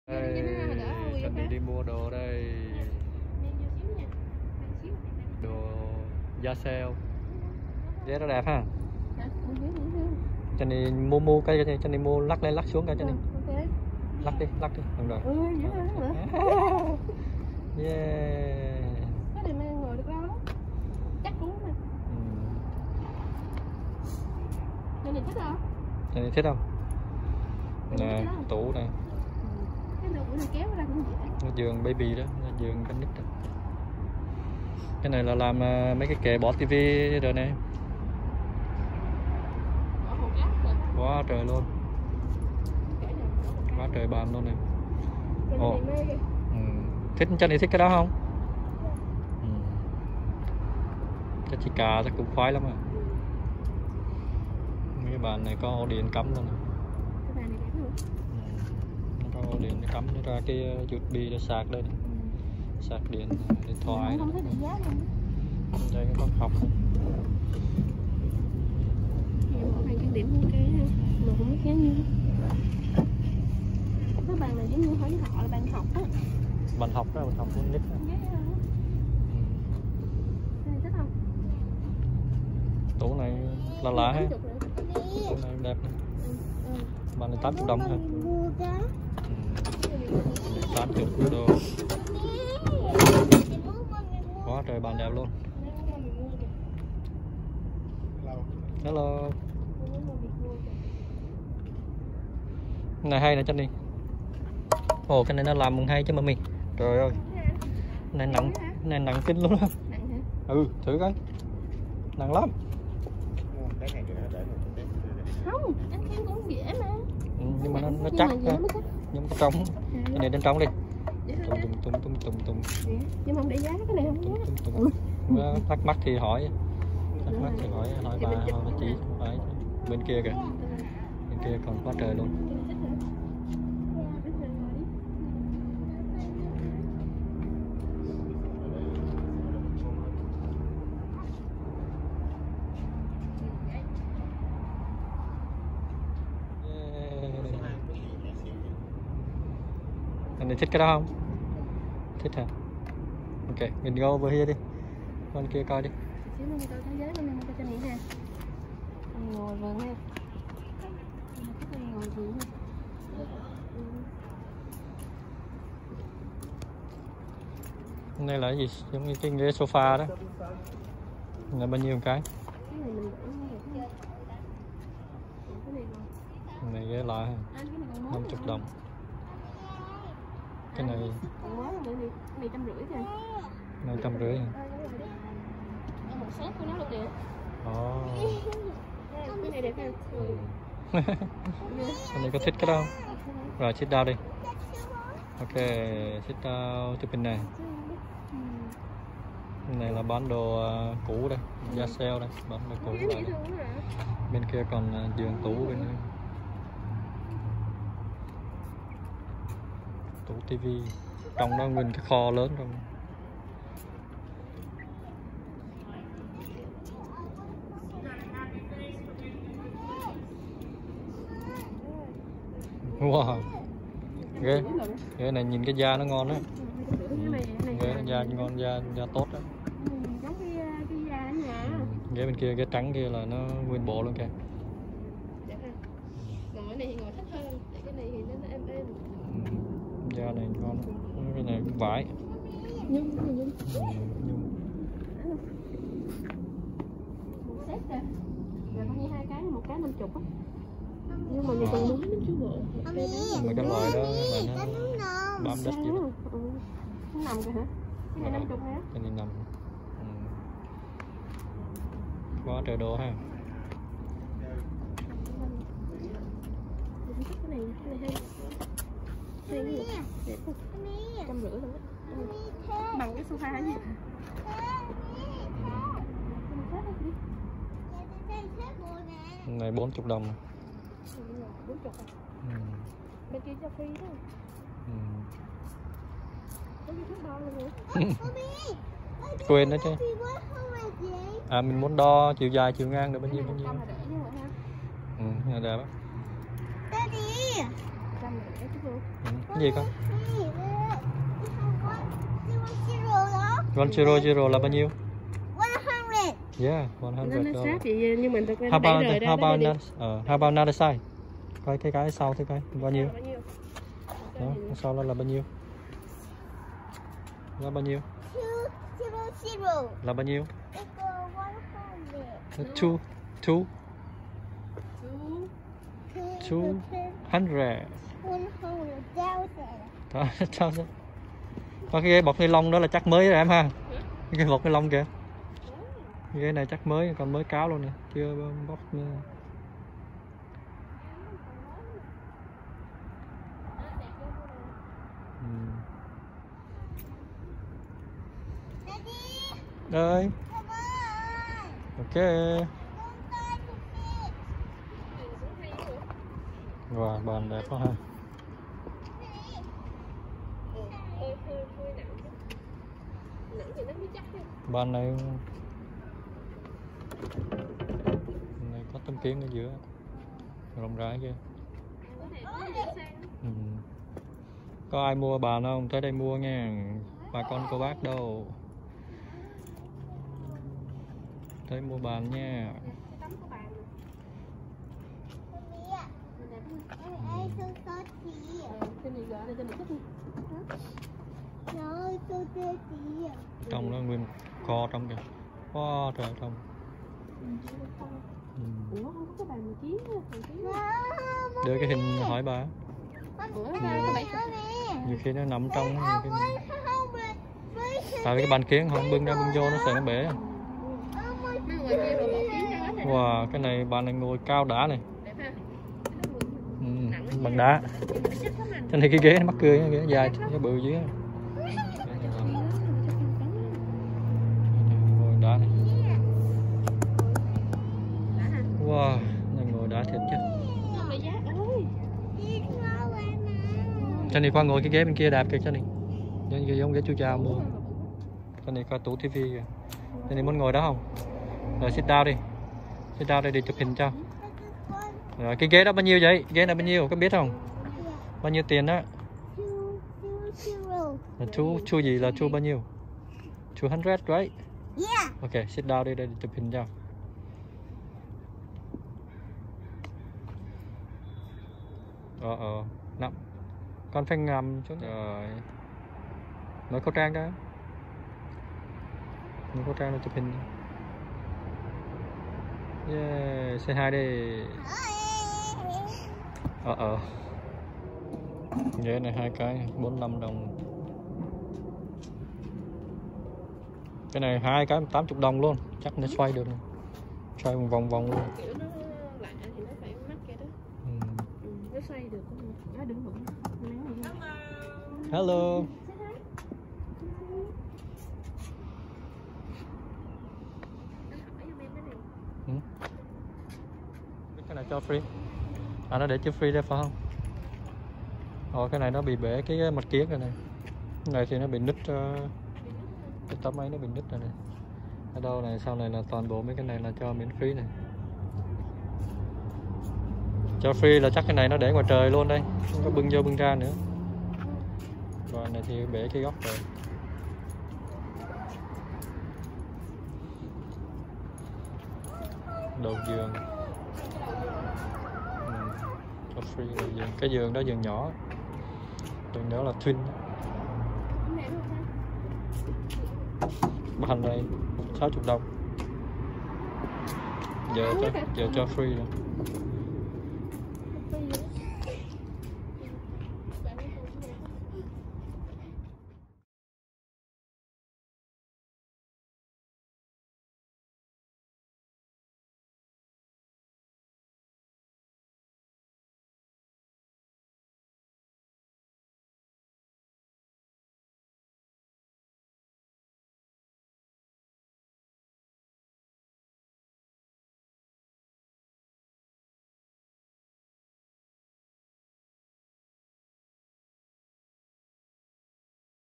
Johnny Đi Mua Đồ Chợ Trời Ở Mỹ - Đồ Nội Thất - Yard Sale - Seattle Washington
Address
14500 Inner Urban Ave. S. tukwila